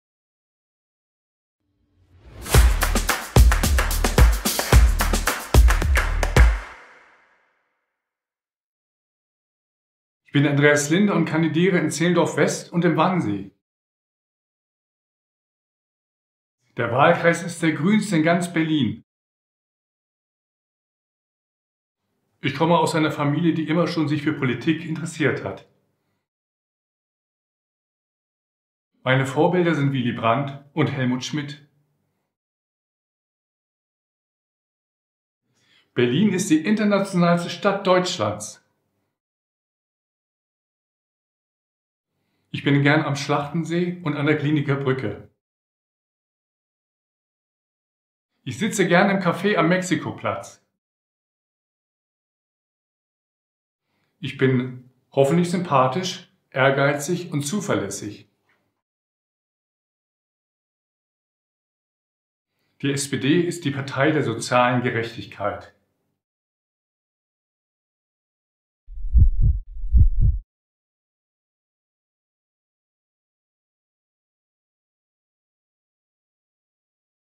Andreas Linde
Andreas Linde kandidiert in Steglitz-Zehlendorf für das Abgeordnetenhaus im Wahlkreis 7.